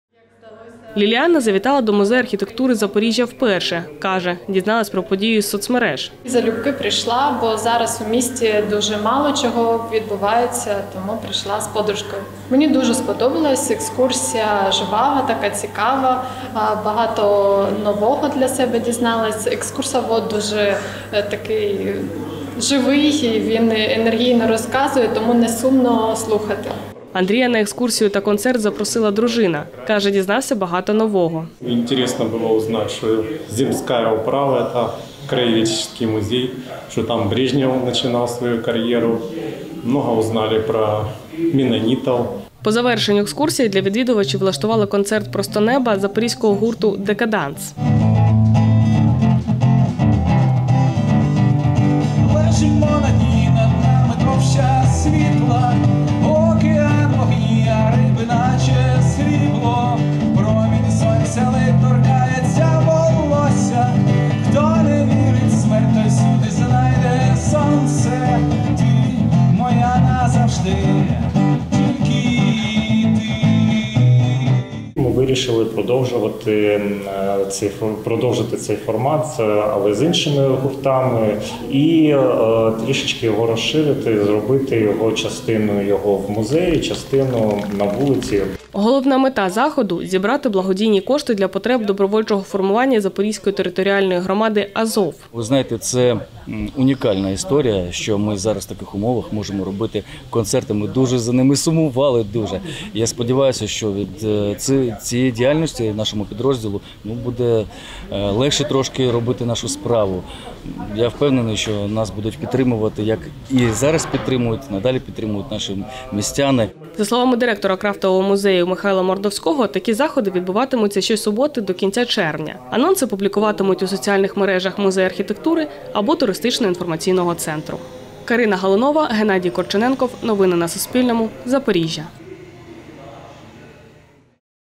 У Запоріжжі під час екскурсії збирали кошти для добровольців | Новини
Благодійний захід для збору коштів на користь добровольчого формування Запоріжжя відбувся у запорізькому музеї архітектури. Люди могли прослухати екскурсію з історії архітектури міста, а також послухати музику у виконанні місцевого виконавця. Як це було бачити кореспонденти суспільного.